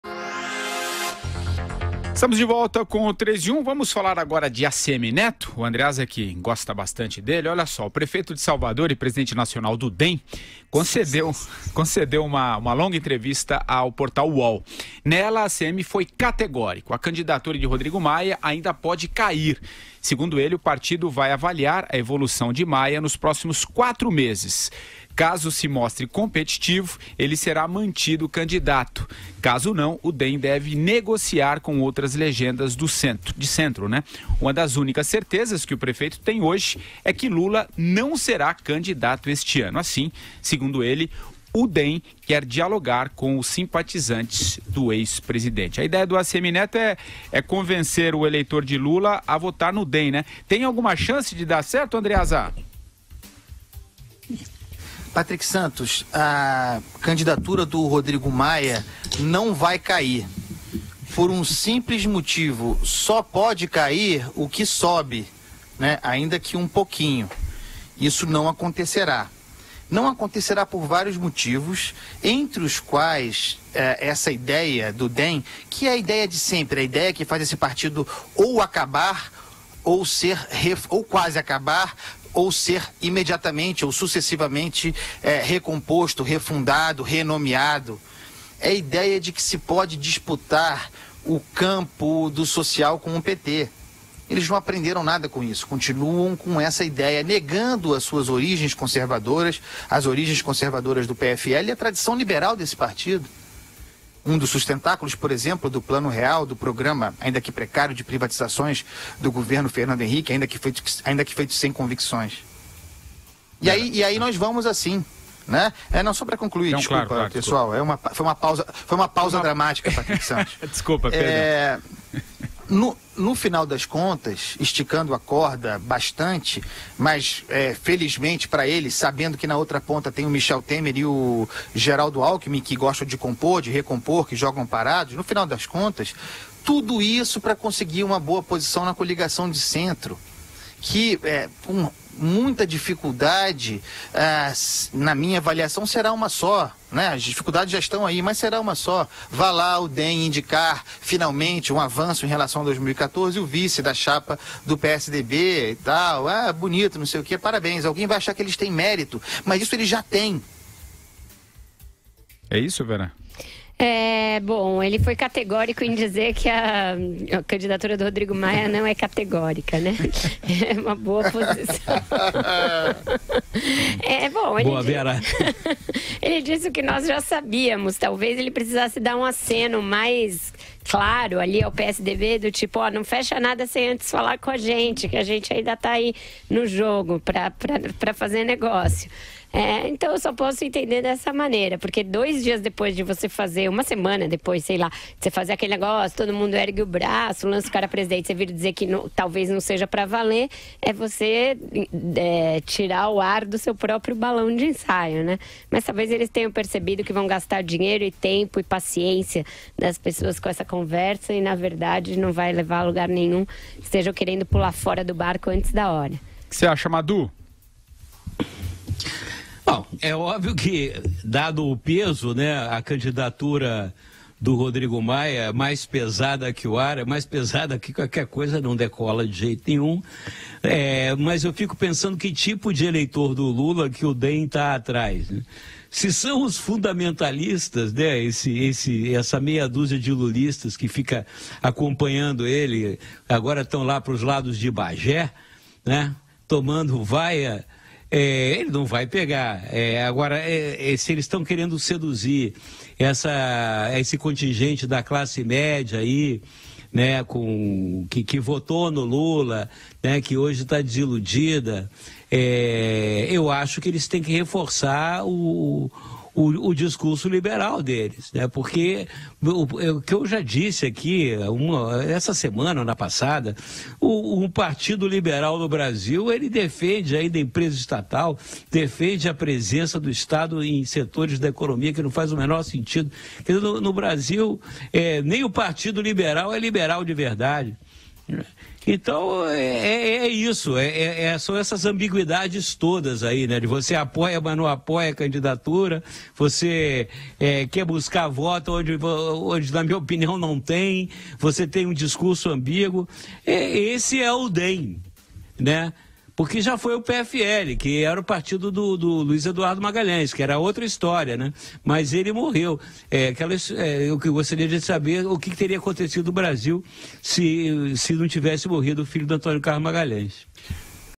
ACM Neto ao UOL: DEM pode retirar candidatura de Maia
Prefeito de Salvador afirmou que vai esperar os próximos meses antes de tomar uma decisão.